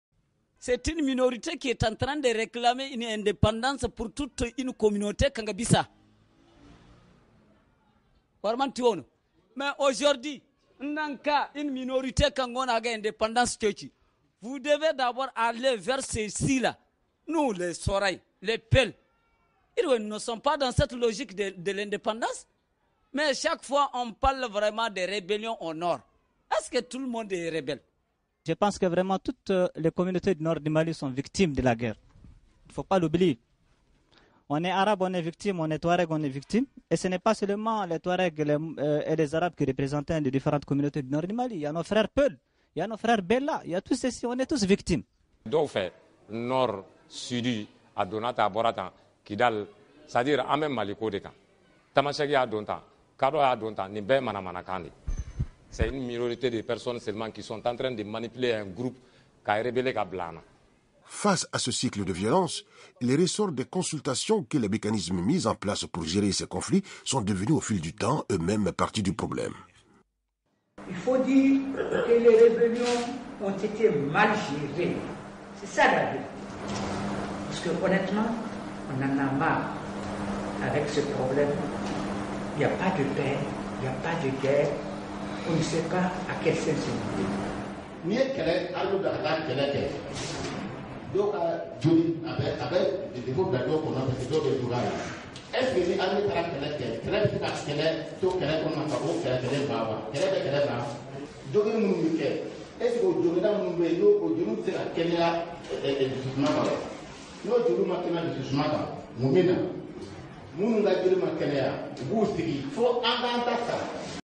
Les rébellion au Mali
Alors d’où vient nos problèmes du nord, les nordistes disent qu’ils y a pas de guerre